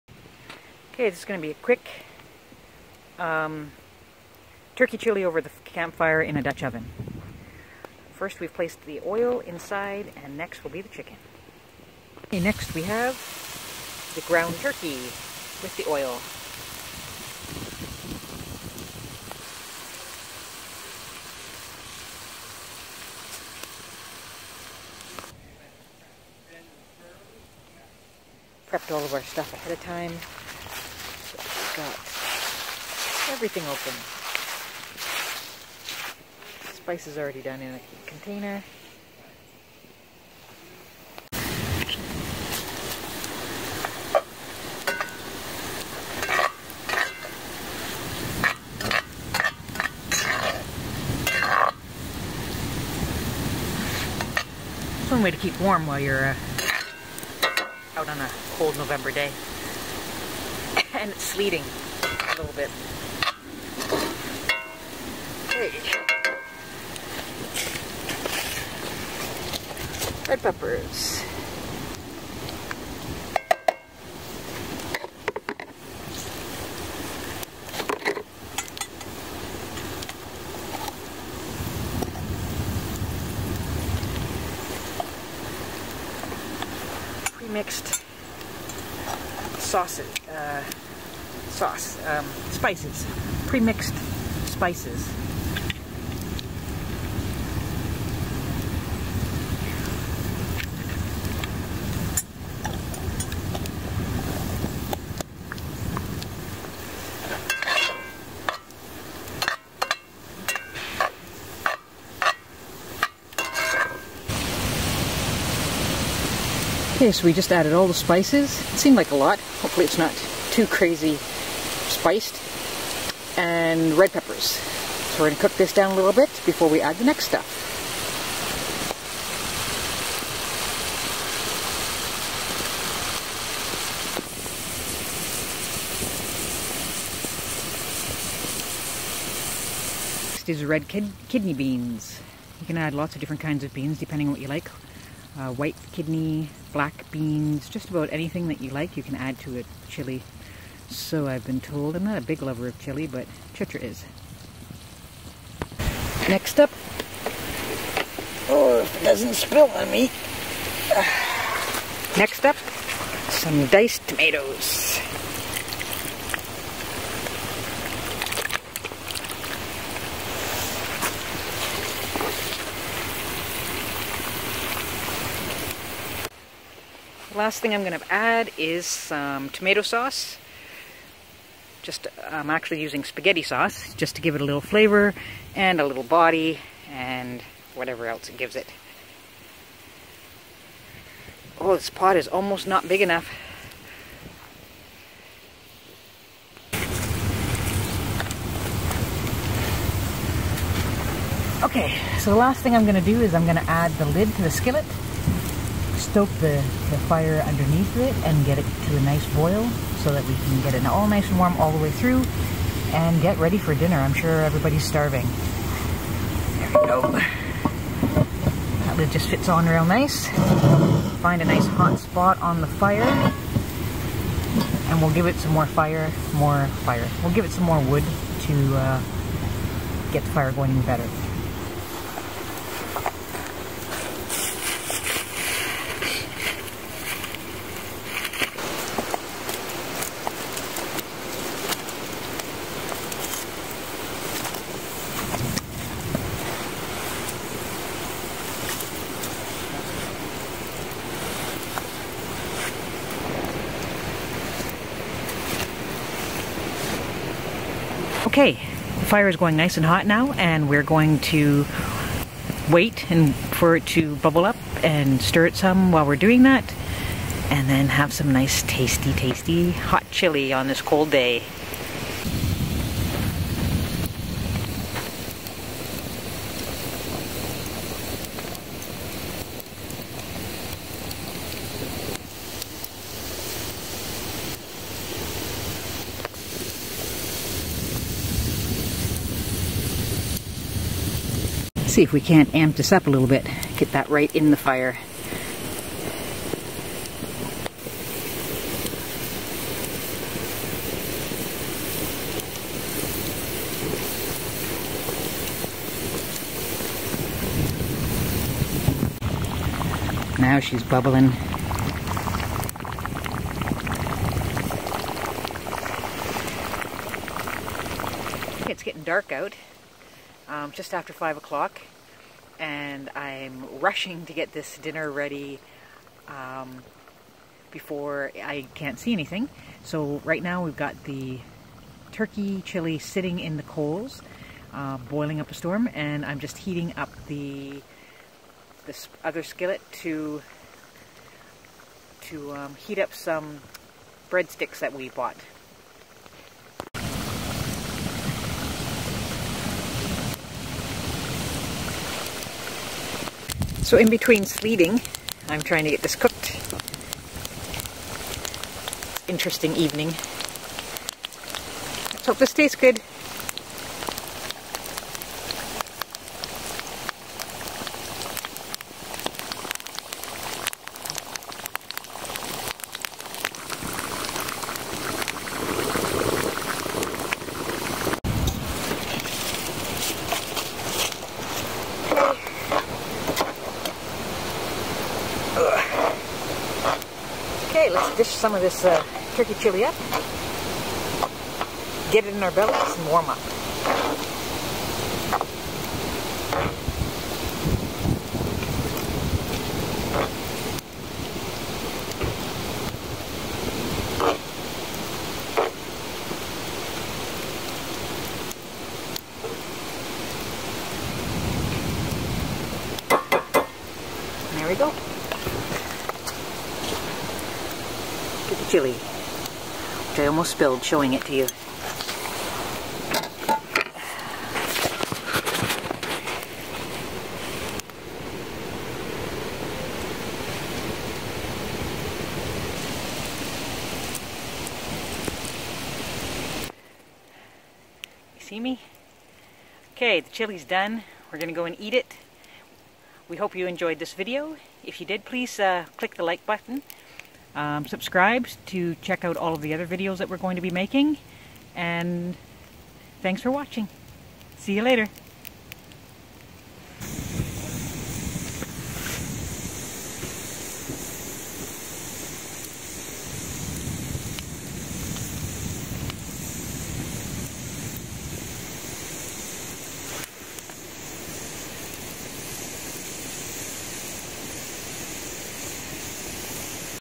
Turkey Chili Over The Campfire At The Pinery Provincial Park - Cooking With Fire Series
On a cold, sleeting autumn evening, I make some homemade Turkey Chili over the campfire. After a cold, wet day of hiking, there's nothing better than a 'stick-to-your-ribs' meal to warm up. We prepped all of the ingredients ahead of time to make the cooking process as quick as possible.

Ground turkey, red bell peppers, spices, beans, tomatoes and some spaghetti sauce make for a delicious and hearty dinner. What's even better is all of the leftovers for lunch and dinner during the week!

Join me as I take you through the steps to make this fantastic chili while I fend off the sleet. I paired the turkey chili with some store-bought garlic bread strips which I heated and crisped on a cast iron frying pan!

Give us some suggestions for future recipes to make while camping. The camping season is upon us and we're always looking for new and delicious meal ideas!